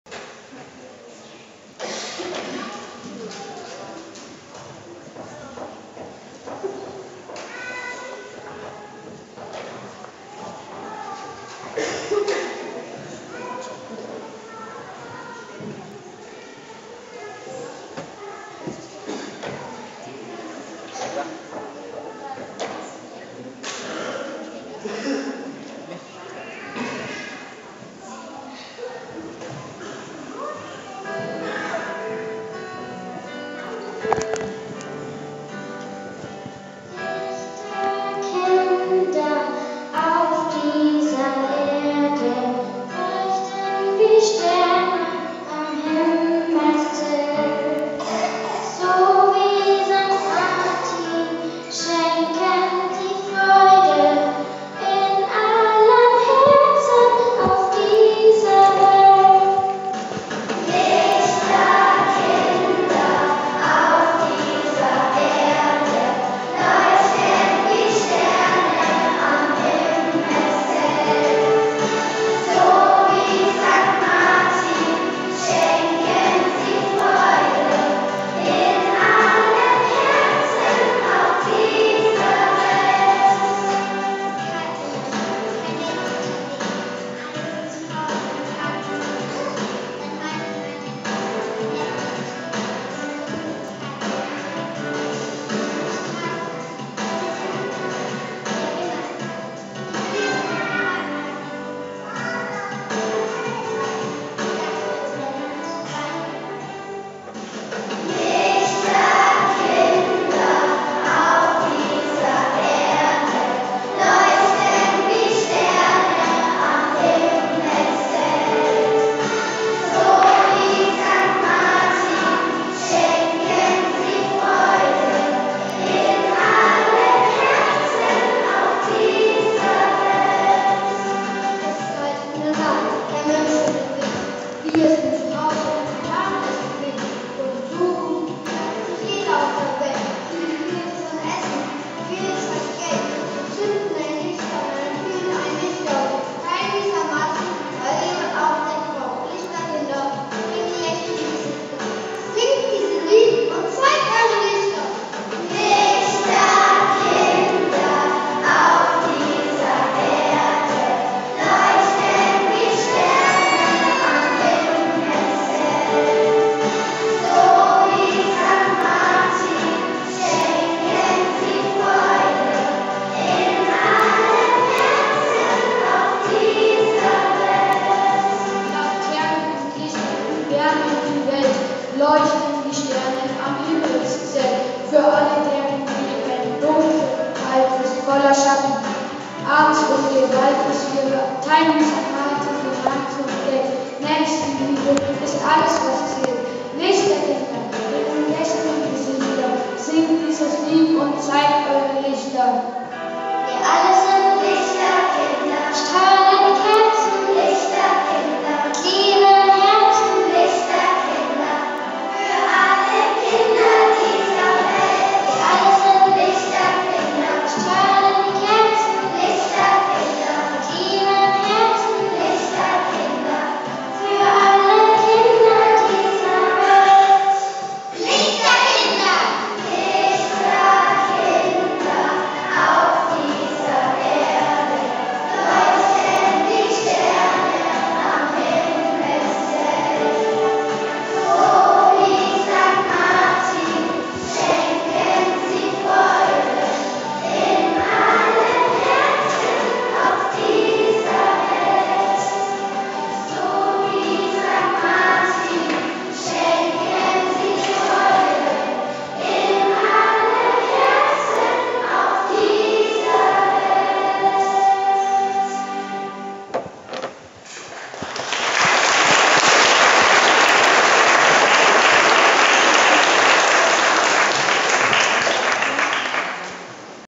Lichterkinder
Am 11. November 2015 gestalteten die Schulkinder der evangelischen Kindertagesstätte Oberkotzau einen Teil zum Gottesdienst zu Sankt Martin.